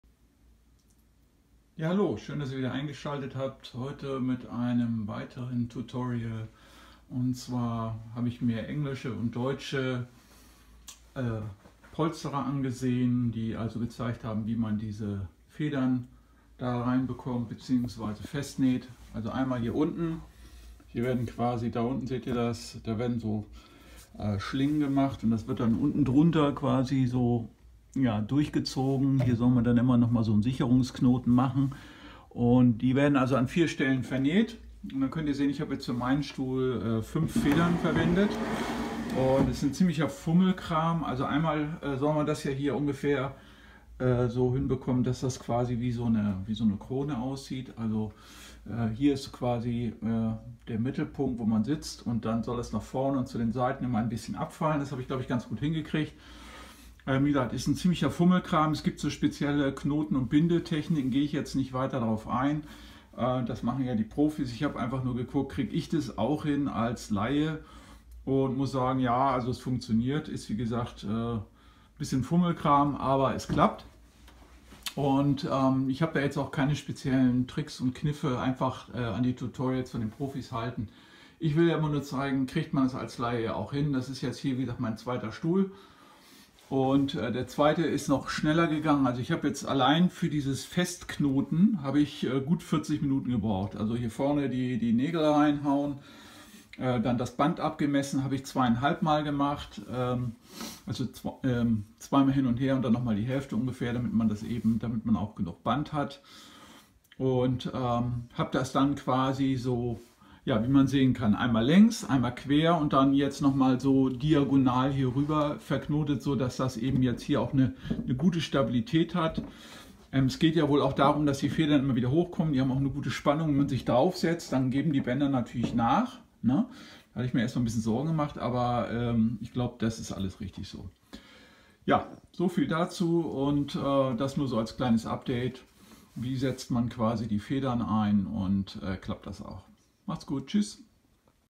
Biedermeier Stuhl Polsterung einbauen
Hier zeige ich eine fertige Knotentechnik zum Einbau von 5 Polsterfedern. Dauert 40-45 min und zusammen mit dem Festnähen der Federn insgesamt ein Stunde.